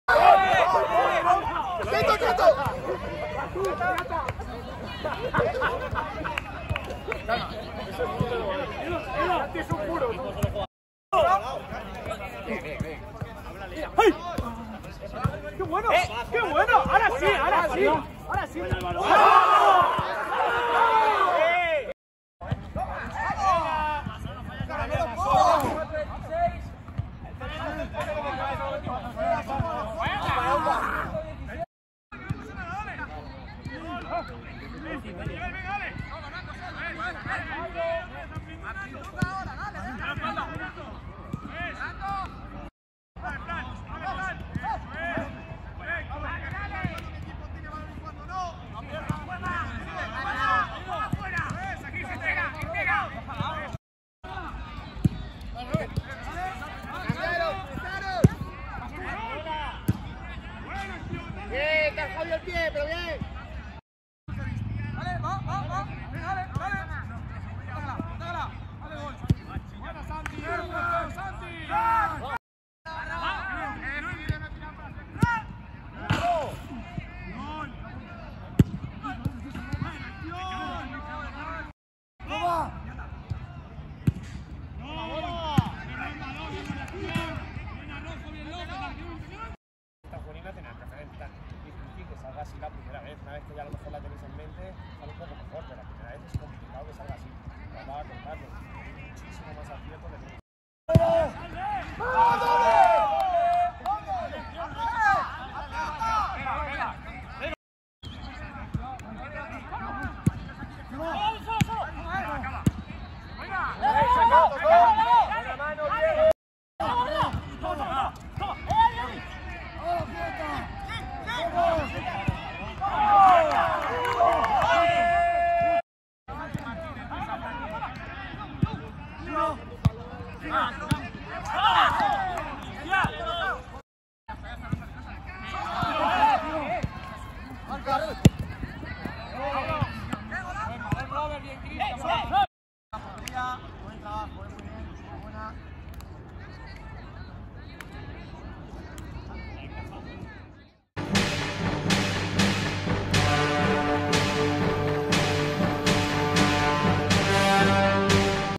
ENTRENAMIENTO SÉNIOR MASCULINO #10: 15/10/22
Los pupilos de Adrián Sotelo han finalizado la semana de entrenamientos con una sesión entretenida y productiva de cara al encuentro del domingo ante el Mejoreño.
🎥🎤: Miguel Cazorla
¡HAZTE SOCIO!